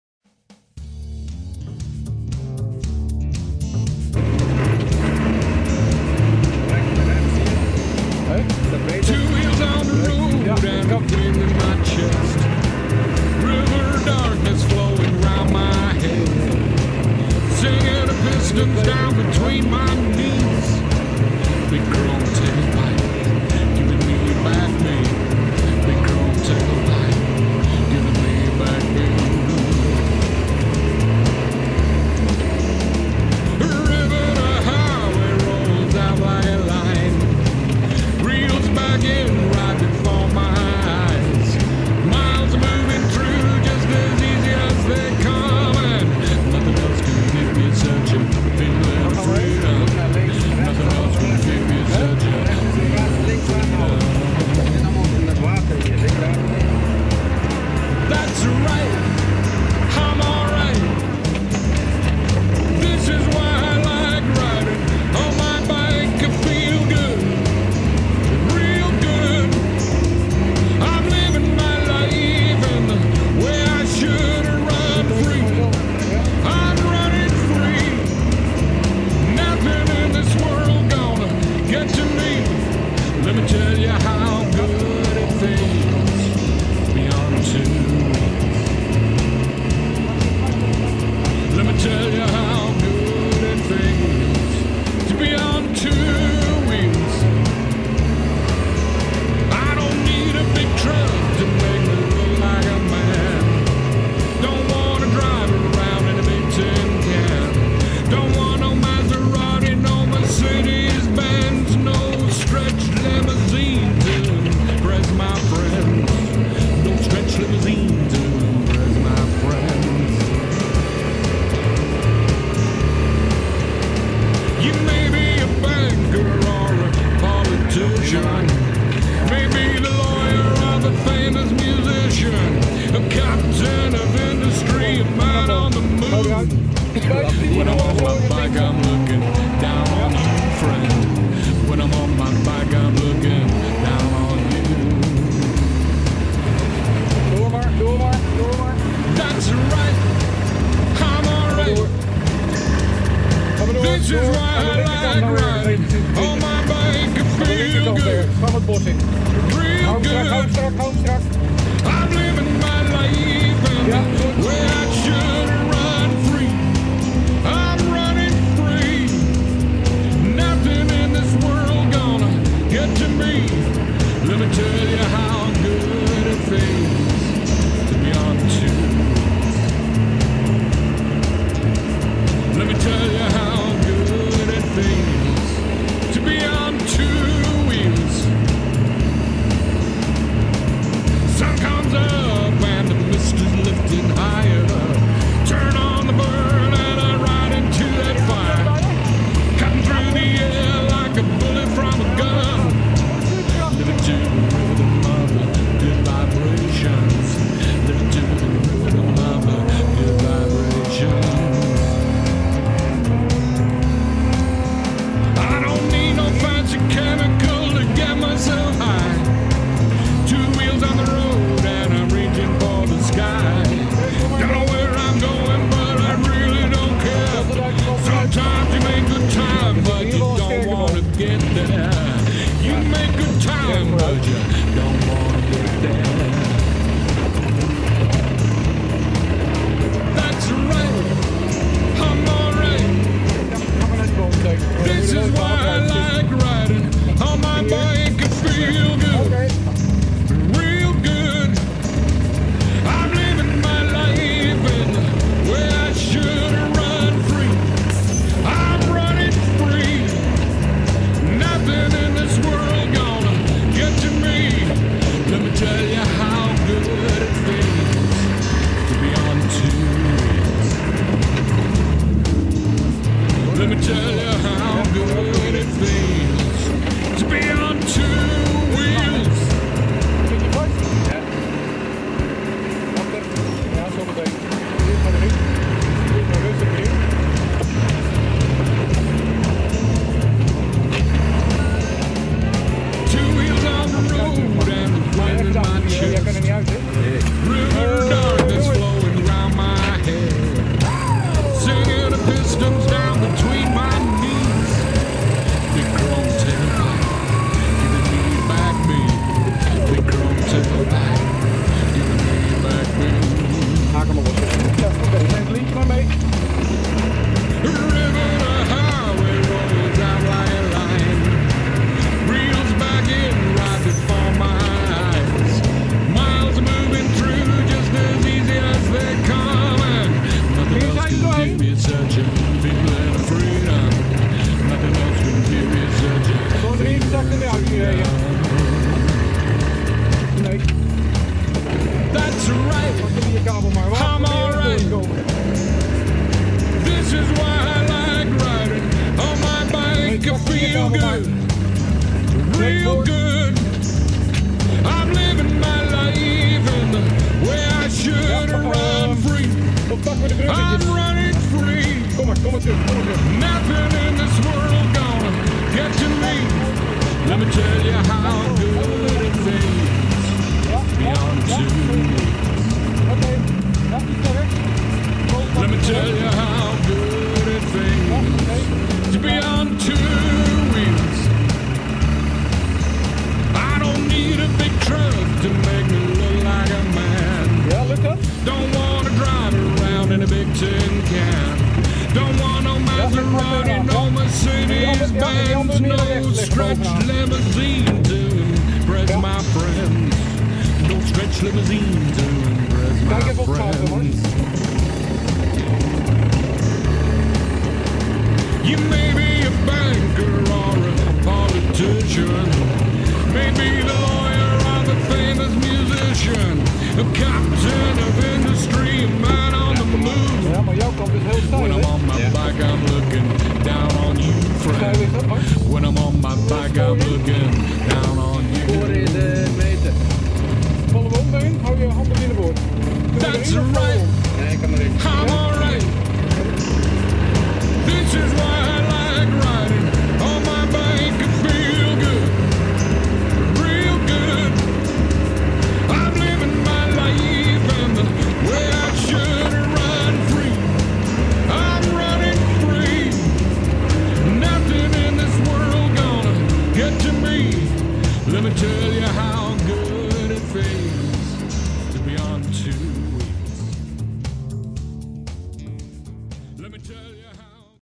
Croatia Trophy 2010 dash-cam (stage 1, part 2)
Croatia Trophy 2010 Stage 1. This section is at the start where we follow a creek for a couple of kilometres. The video camera is mounted on the dashboard (before GoPro's) to try and get a feel of what it's like INSIDE the car...

A baptism of fire for the uninitiated. Plenty of mud after the rains and a section through the creek that was carnage...